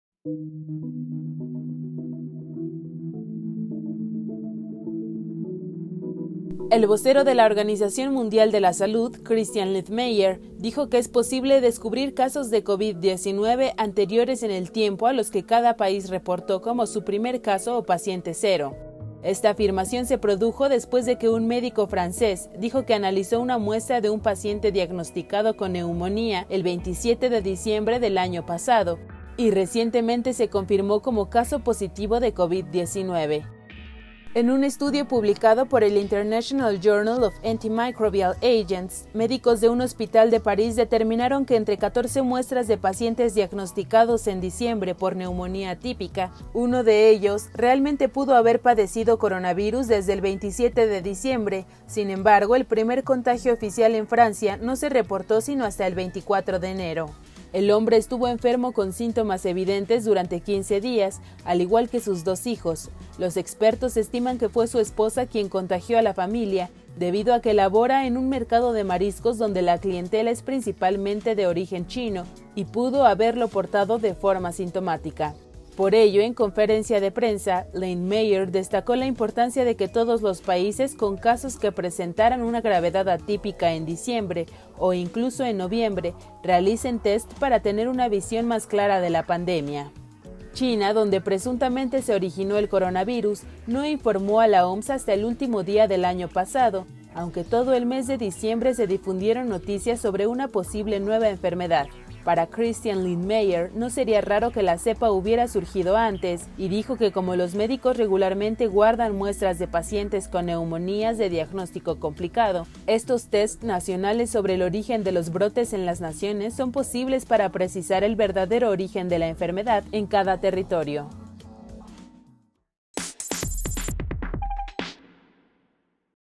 Vocero de OMS pide rastrear primeros casos de COVID-19
El vocero de la Organización Mundial de la Salud, Christian Lindmeier dijo que es posible descubrir casos de COVID-19 anteriores en el tiempo a los que cada país reportó como su primer caso o paciente cero.